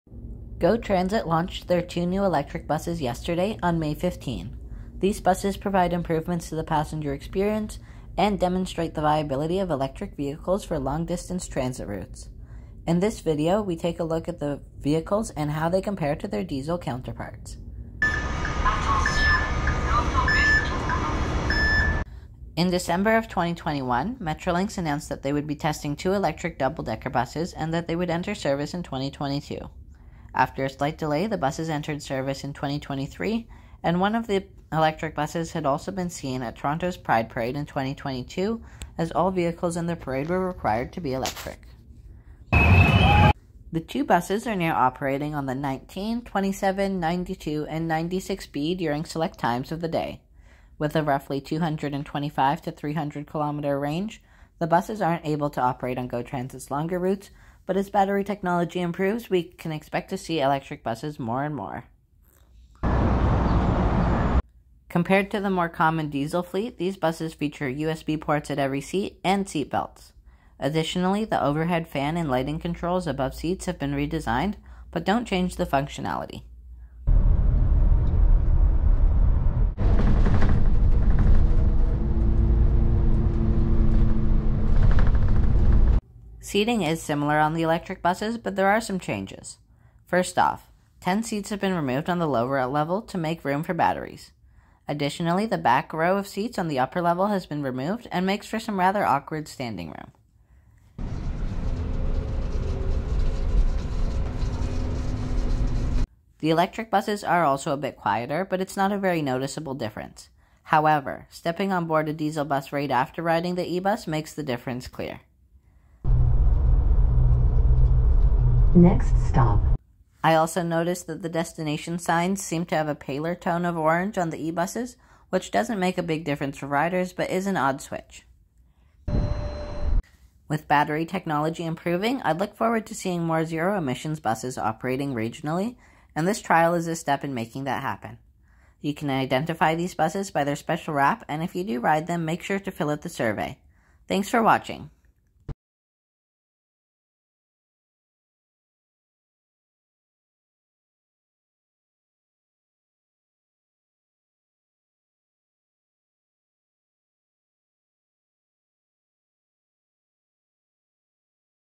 GO Transit's New Electric Buses Are In Service!
In today's video we take a look at the new e-buses being operated by GO Transit on a few of their routes. These new buses feature better amenities and will contribute to bringing electric buses to long distance routes.